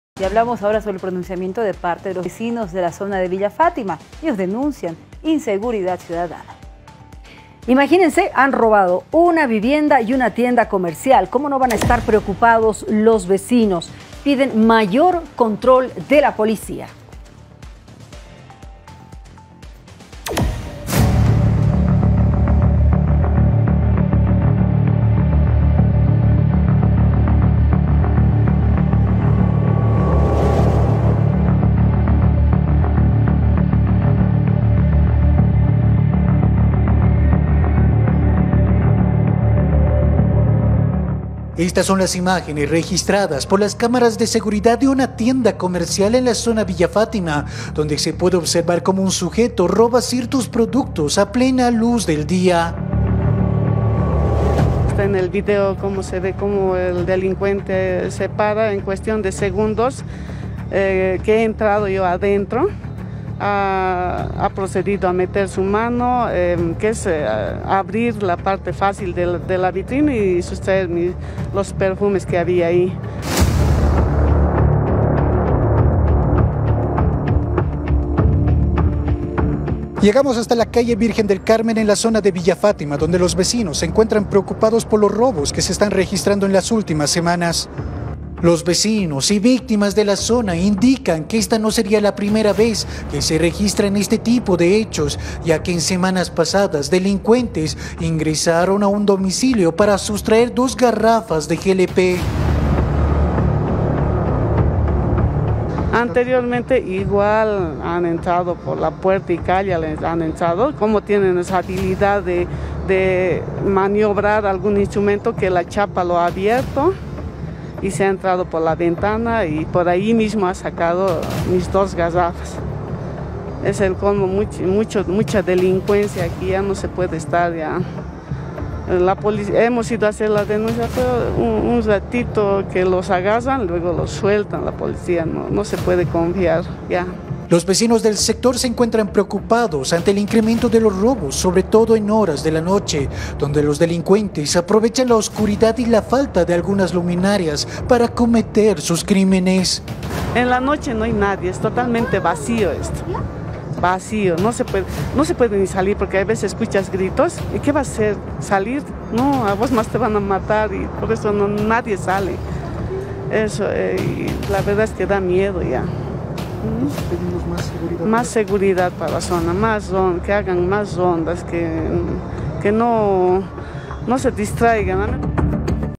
Roban a una vivienda y una tienda comercial en Villa Fátima. Vecinos piden mayor control policial
Roban a una vivienda y una tienda comercial en Villa Fátima. Vecinos piden mayor control policial ante inseguridad.